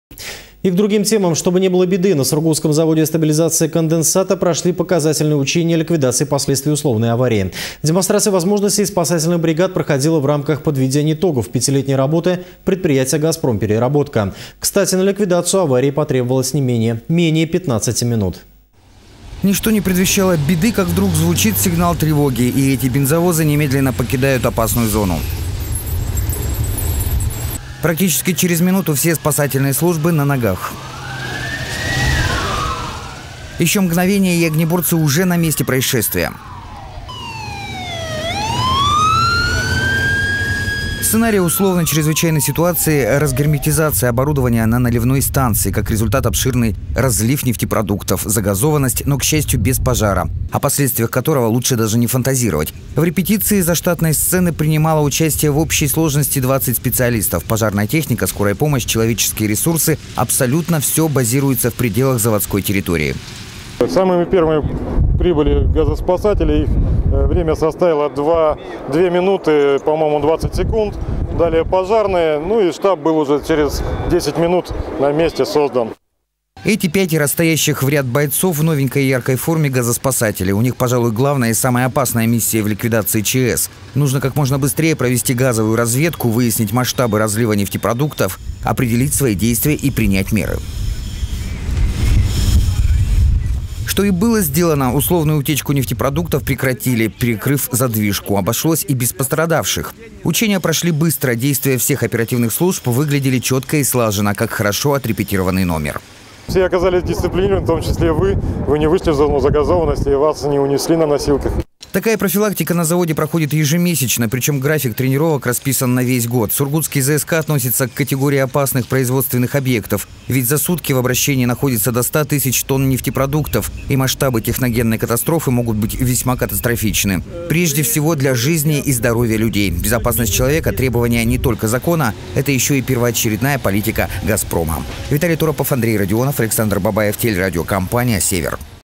Чтобы не было беды
На Сургутском заводе стабилизации конденсата прошли показательные учения ликвидации последствий условной аварии. Демонстрация возможностей спасательных бригад проходила в рамках подведения итогов пятилетней работы предприятия «Газпром - Переработка».